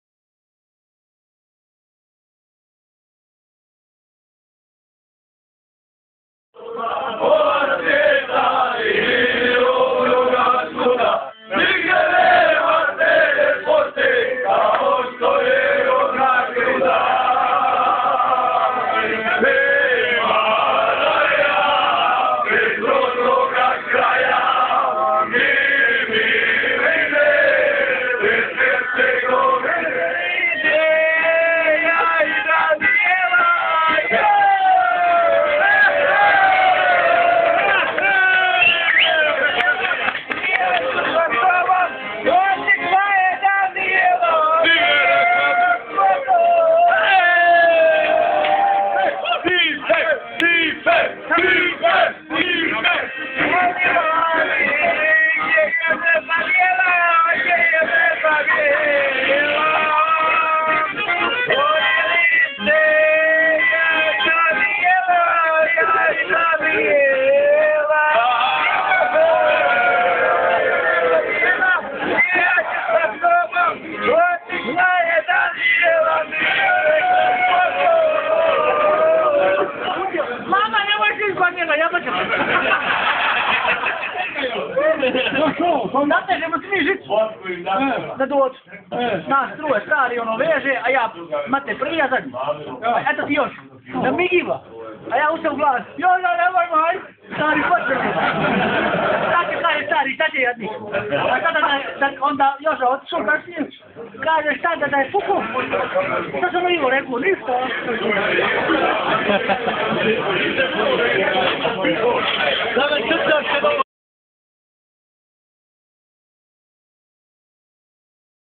Best of Ćaić City
Malo smo se potrudili,pa smo odvojili najuzbudljivije trenutke sa fešte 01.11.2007.
Posjetite nas
caiccity.blog.hr